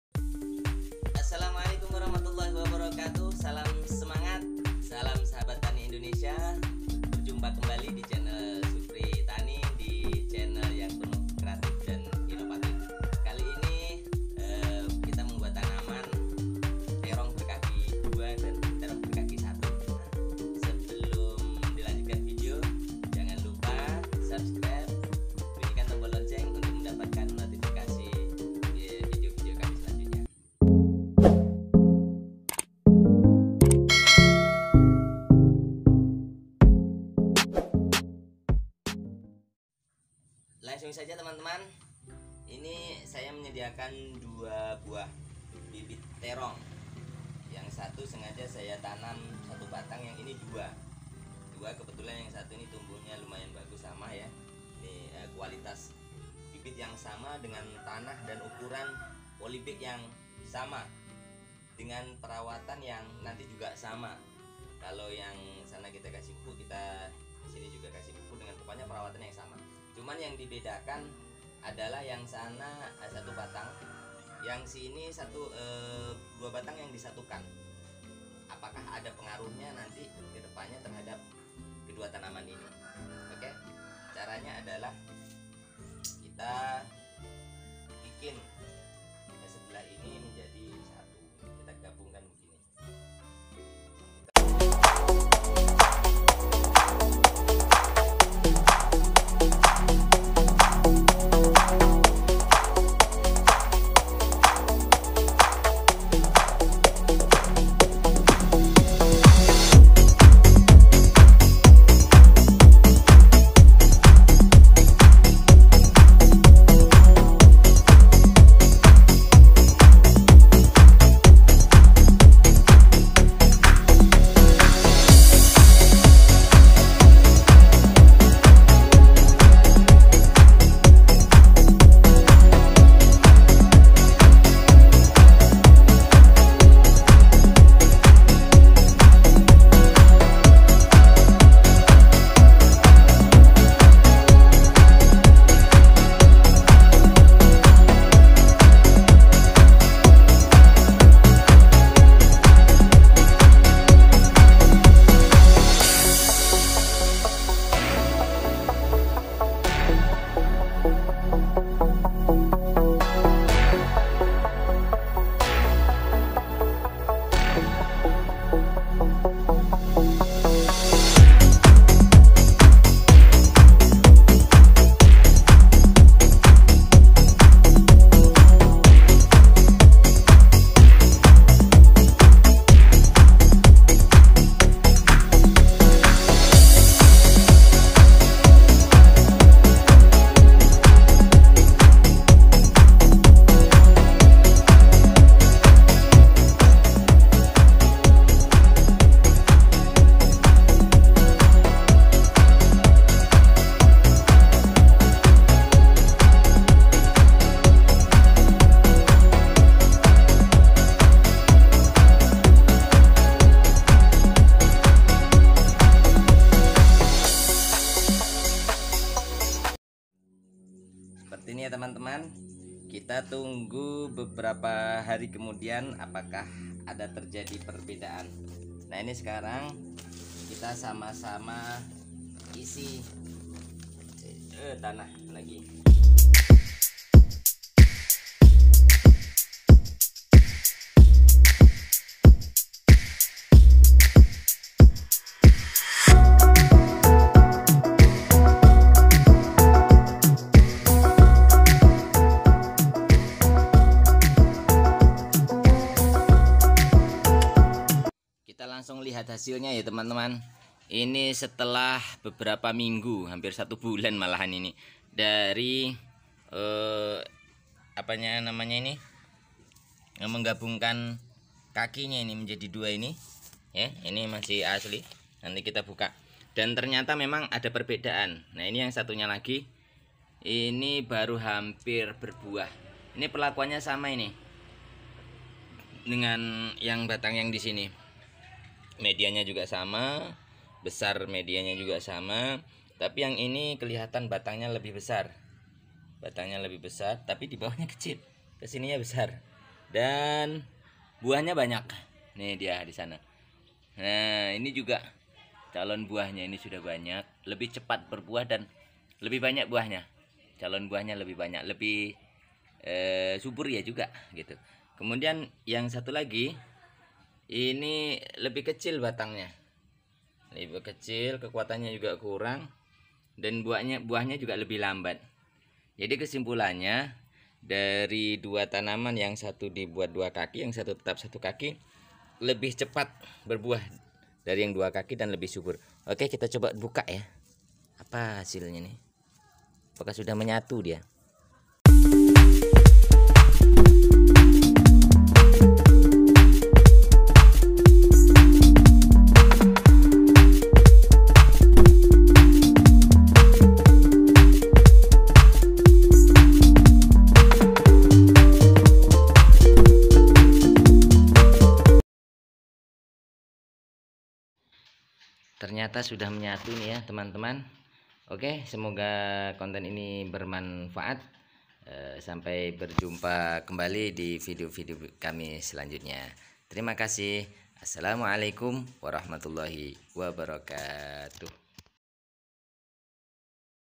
BUDIDAYA TERONG UNGU || SATU KAKI VS DUA KAKI (mana lebih bagus????)
Budidaya terong ungu dengan satu kaki dan dua kaki mana lebih bagus?